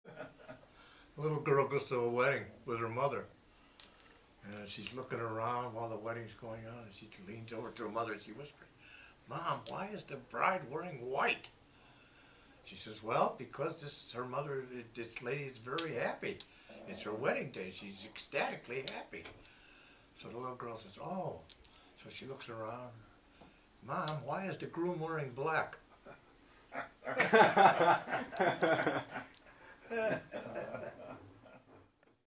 Three Men Sit Around Telling Jokes 2
Los Angeles Jewish Home residents Ellis Simon (87), Bert Harrison (92), and Shelly Balzac (83) sit around in one of the conference rooms telling jokes to one another.